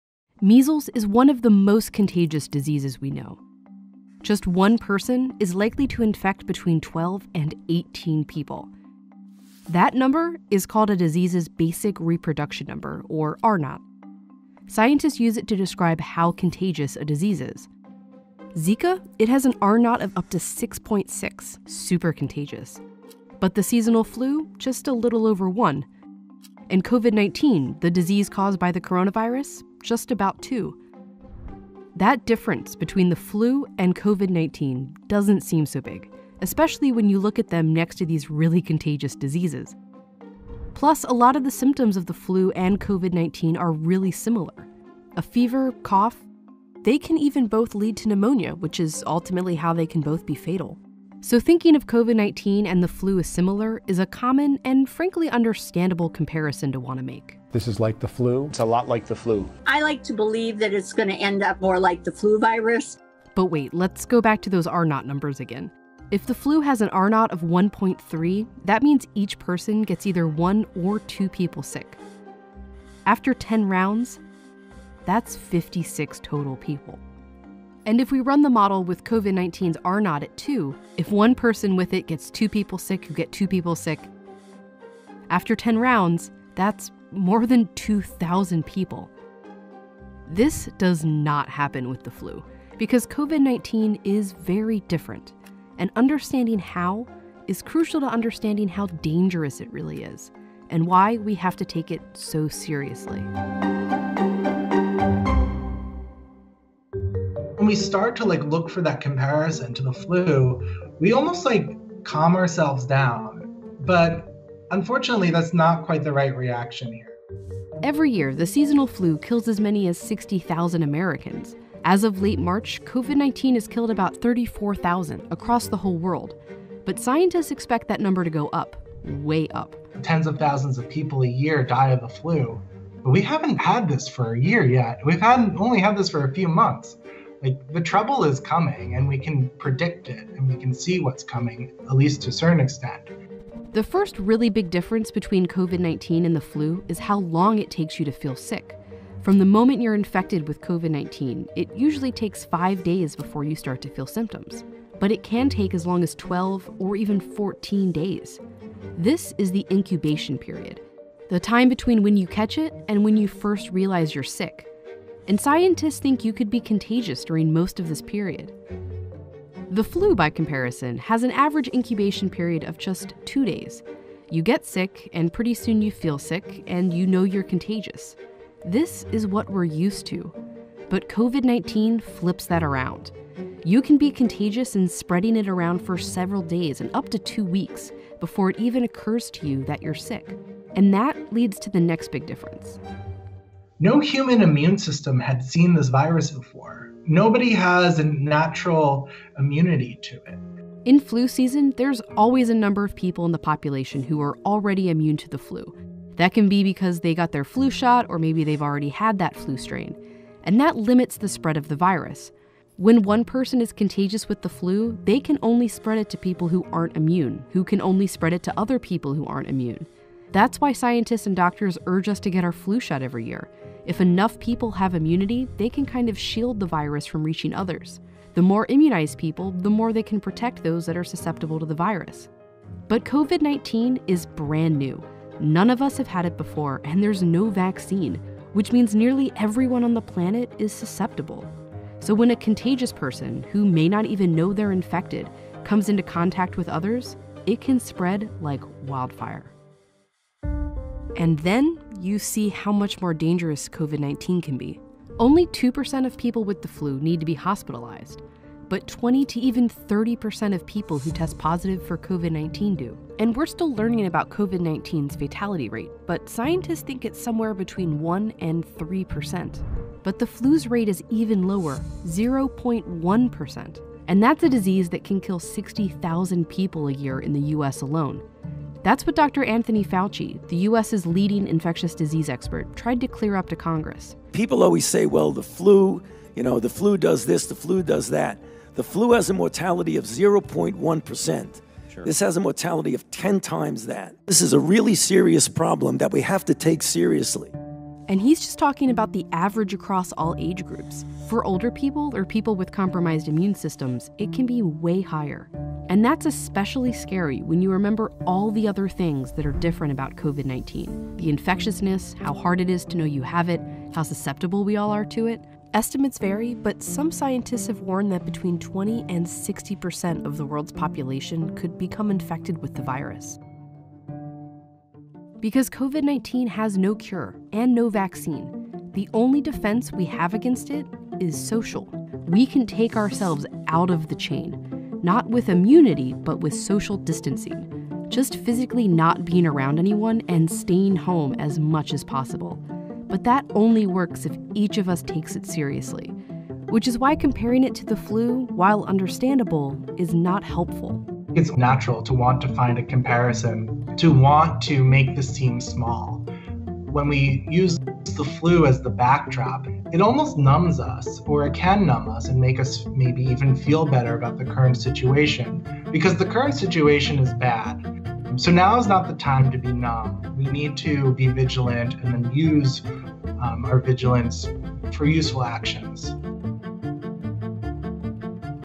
新型コロナウィルスとインフルエンザは何が違うか？
米メディア「Vox」がまとめた「新型コロナウィルスとインフルエンザ」の違いがとてもわかり易かったので、Youtube版のサブタイトルではない字幕版を作成してみました。